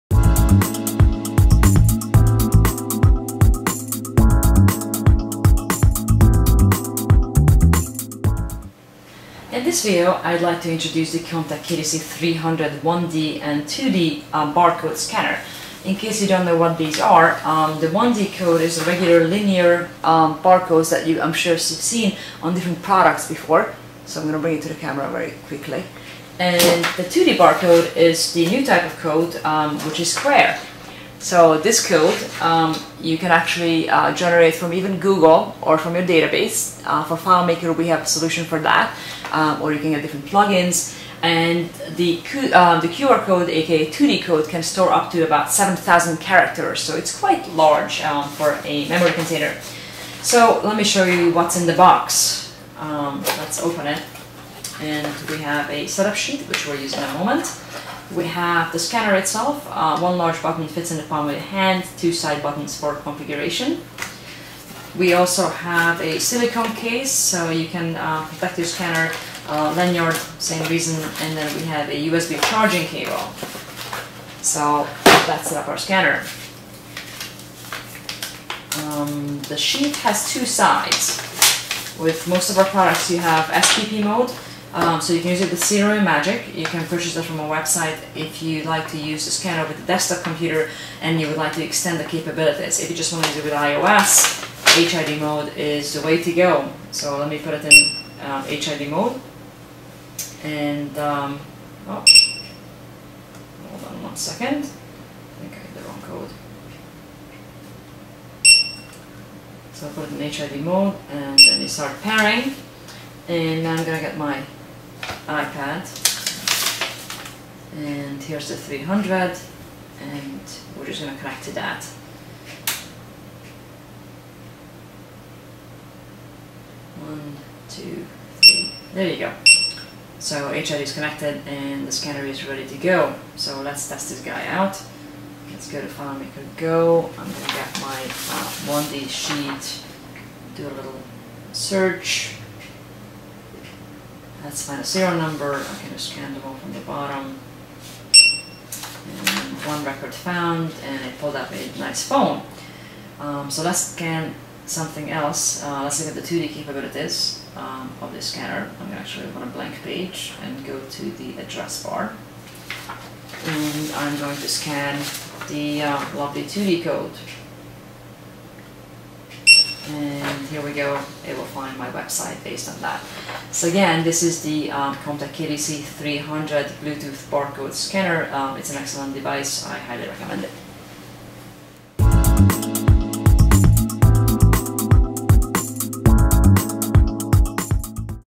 Koamtac KDC 300 Bluetooth 1D and 2D Barcode Scanner
This video demonstrates how to pair the KDC 300 scanner with an iPad. We use FileMaker to test the scanning. The KDC 300 is a tiny yet robust barcode scanner that can scan linear, as well as 2D (QR) codes. It can be paired with any device that has a Bluetooth chip in it. It works out of the box with desktops and mobile platforms such as Android and iOS devices. The keyboard can be used on the iOS devices by pushing the down button on the side of the scanner.